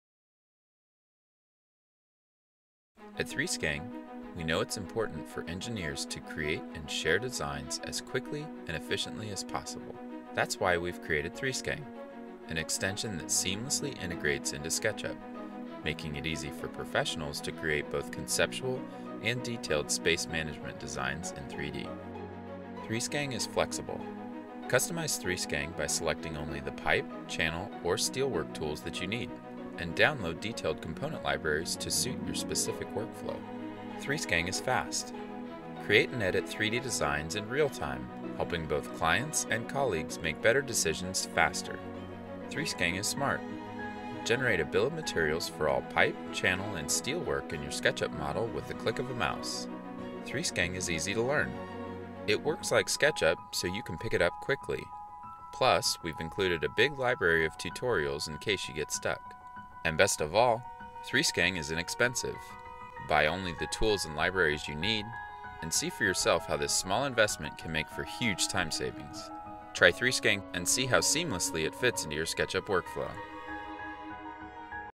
3skeng tools for Trimble SketchUp (with english subtitles)
Promo video of the 3D Engineering extensions “3skeng” “List”, "Pipe", "Mount", "Steelwork" and "Channel" for Trimble SketchUp that are developed by SHK, a software development, engineering and consulting company.